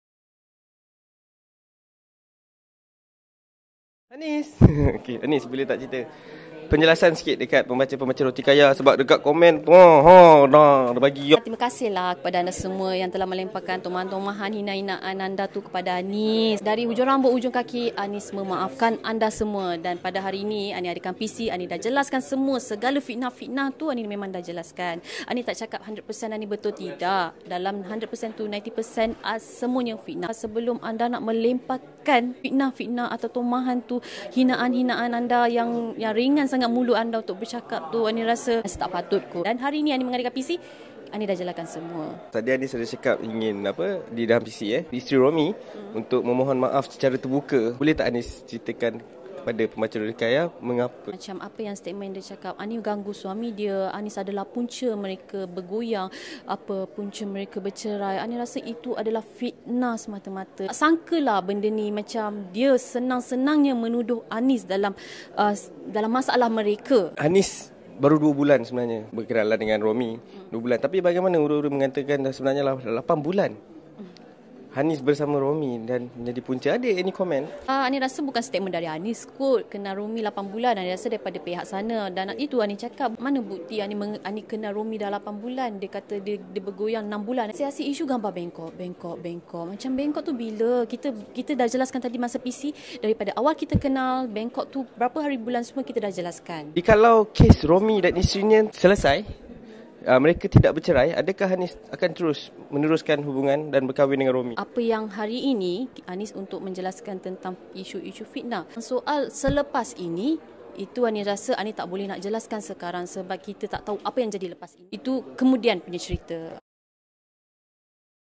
Hanez Suraya: Tak Sangka Isteri Dia Senang Senang Tuduh & Fitnah Saya
Hanez Suraya baru baru ini mengadakan satu sidang media khas untuk menjelaskan yang semua tuduhan dibuat Liyana Manan (isteri kepada Mr R) merupakan fitnah semata mata dan meminta wanita itu memohon maaf secara terbuka.